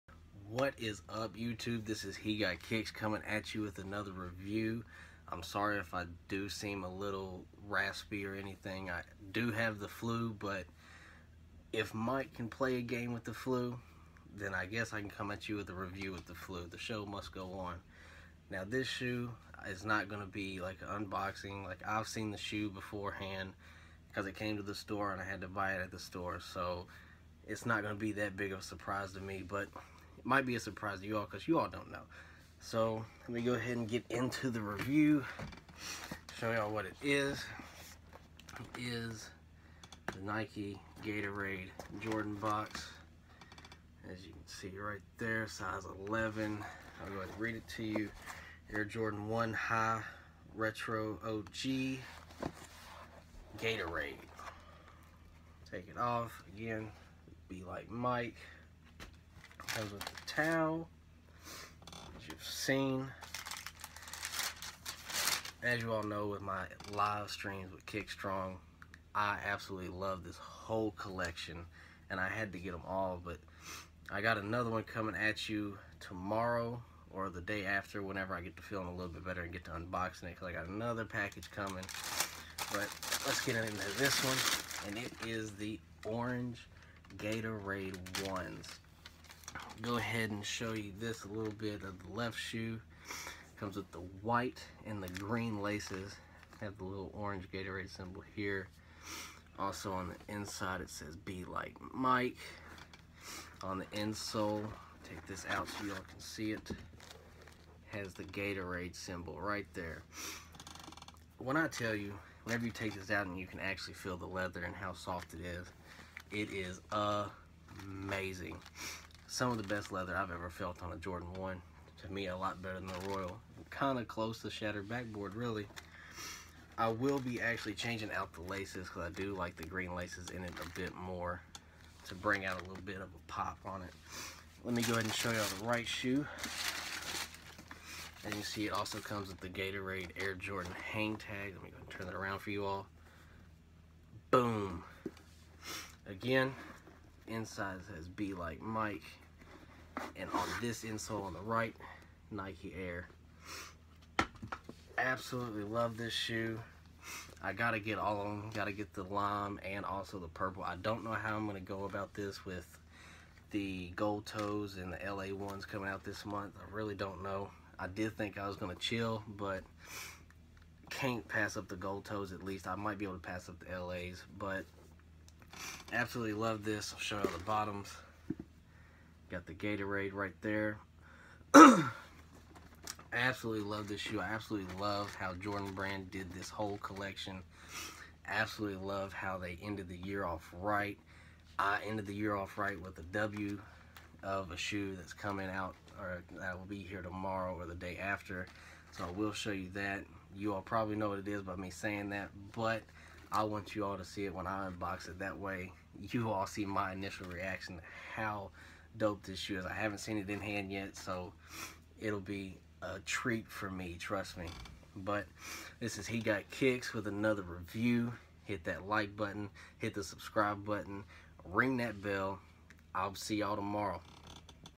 REVIEW OF THE ORANGE GATORADE 1's!!!!
Here is my review of the orange Gatorade 1's out of the like mike collection by Jordan brand! Be ready for another review in the coming days!!